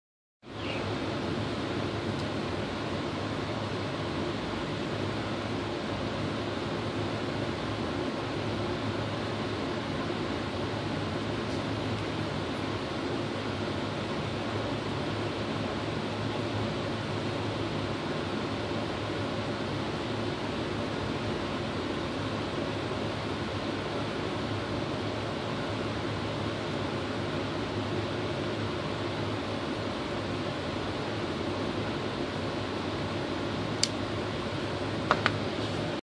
Custom Ghostbusters PKE Meter version 13 lights demo
Demonstration of the lights in my 13th version of custom PKE Meters based on the technology in the Ghostbuster movies.